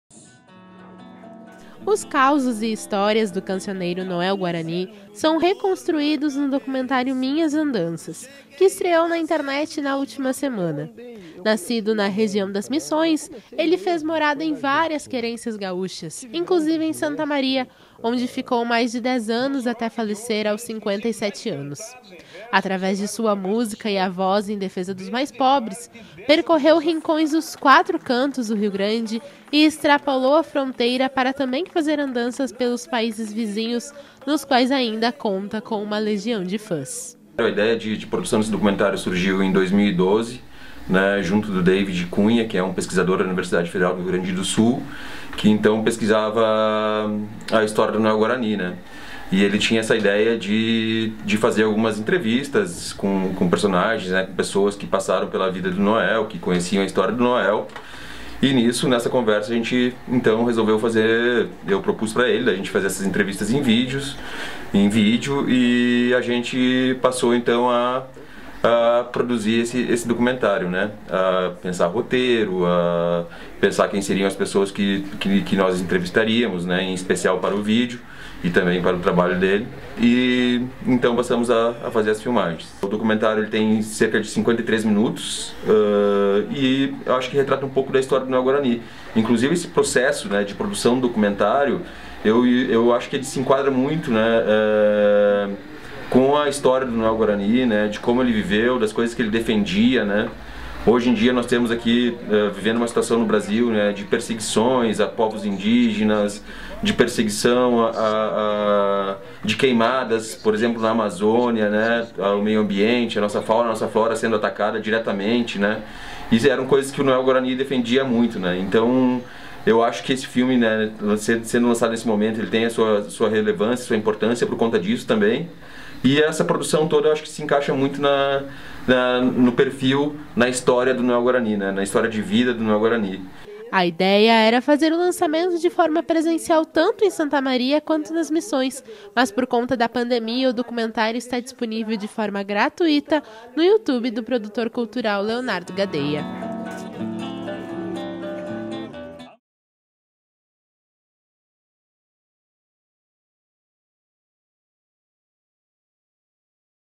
Documentário reconstrói trajetória do cancioneiro Noel Guarany
Filme está disponível de forma gratuita no Youtube do produtor cultural Leonardo Gadea